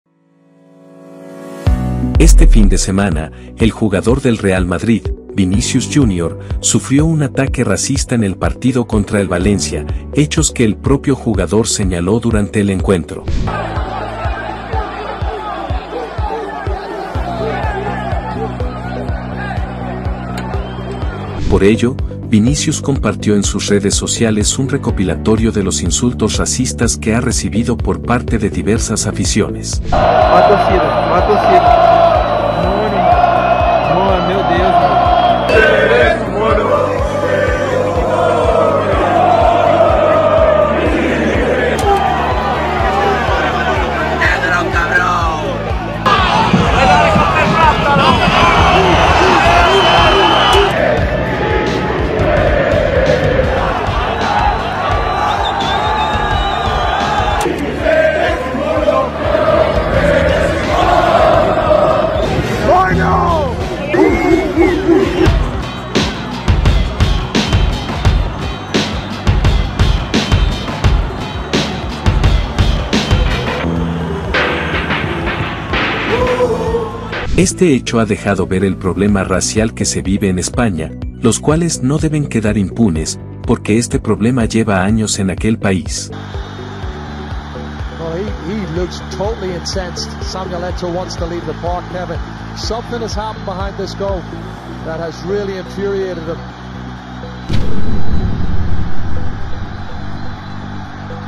El jugador del Real Madrid, ViniciusJr sufrió fuertes insultos en un partido de fútbol.
Este fin de semana el jugador del Real Madrid, ViniciusJr sufrió fuertes insultos en un partido de fútbol. ¡Ya basta de racismo!